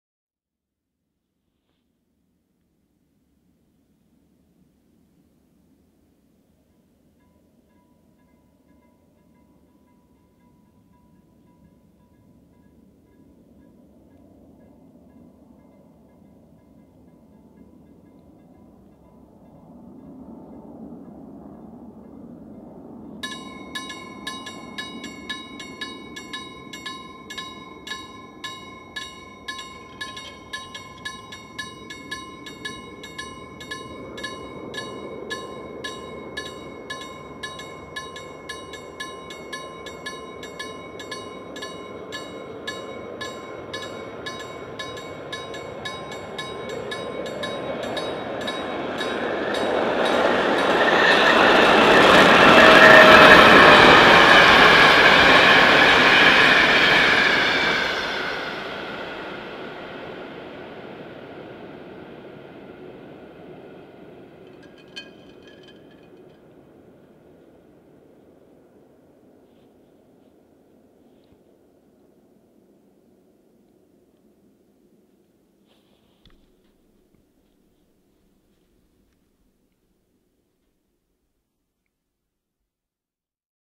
Express train 707 passes Markkala level crossing
Express train 707 from Kouvola to Oulu passes Markkala level crossing at Markkala, Suonenjoki, Finland. Temperature is -20 degrees Celcius, and that gives spectacular arc between catenary and pantograph.